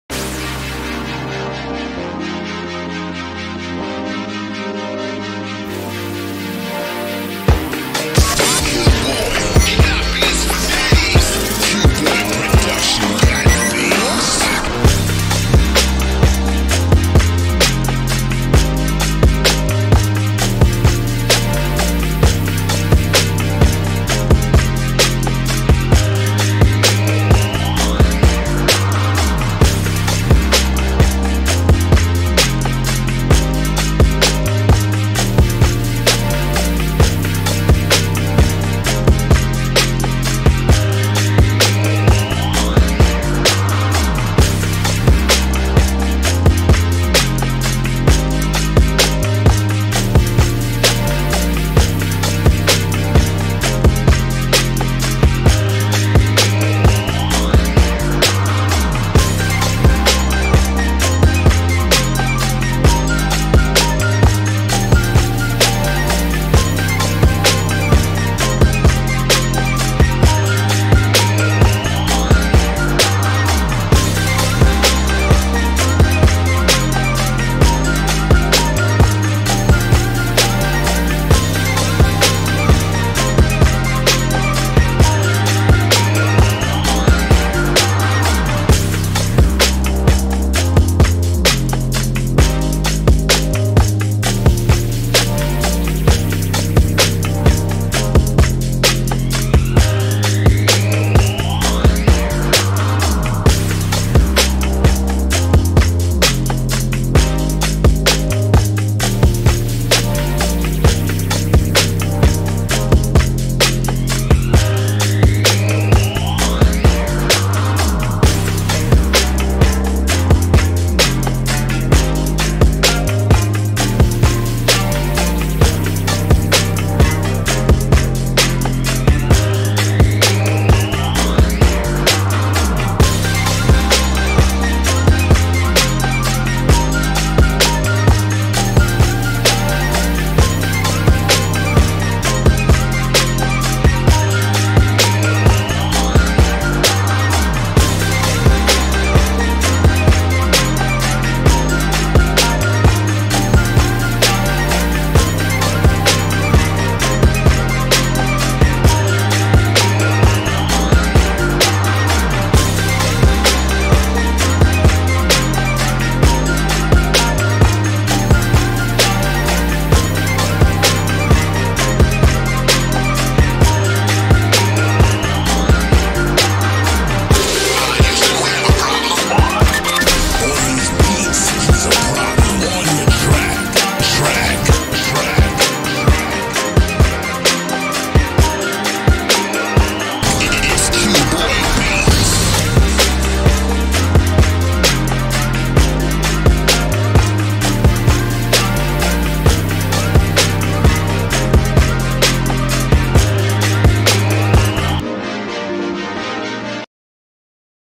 FILM MUSIC SCORE CAR COMMERCIAL qboybeats.com
Instrumentation at it's finest. High Energy  Instrumentation for Motivation.  Great for film score, commercial hood street action,  create, manifest with this work. Light up, close your eyes and enjoy.
FOR LEASING AND/OR EXCLUSIVE RIGHTS PRICING INFORMATION